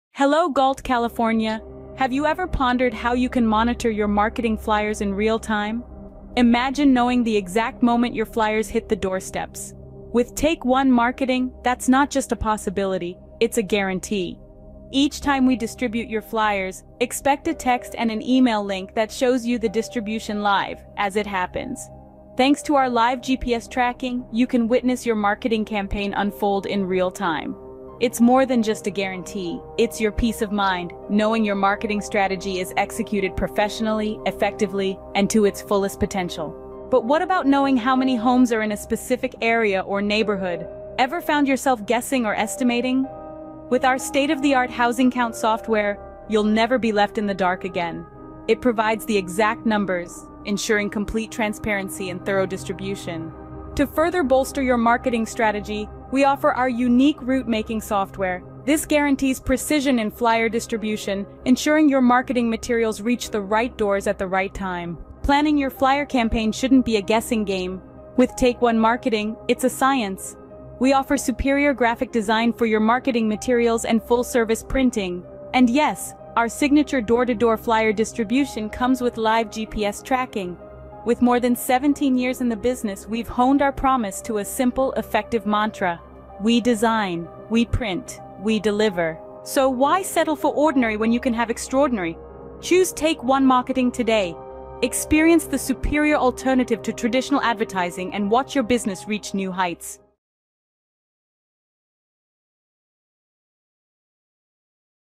Door to Door Flyer Delivery in Galt California By Take 1 Marketing
Discover the transformative impact of Take 1 Marketing's Door to Door Flyer Delivery Services in Galt, California, with our latest video showcase. Experience how our customized flyer distribution campaigns create a direct link between home improvement and renovation businesses and their perfect audience. From the lively avenues of Galt to its serene, suburban areas, our Take 1 Marketing team utilizes strategic planning, targeted precision, and cutting-edge technology to guarantee your marketing message is directly received by prospective customers. This video provides an insider view of our operation, from crafting visually striking flyers to their precise delivery, showcasing our use of live GPS tracking for unrivaled efficiency and accountability. Whether you're a business in Galt looking to enhance your visibility or a homeowner in search of top-notch renovation services, see how our door-to-door flyer delivery can significantly contribute to achieving your objectives. Enjoy testimonials from our happy customers and peek into the evolving landscape of direct marketing in Galt.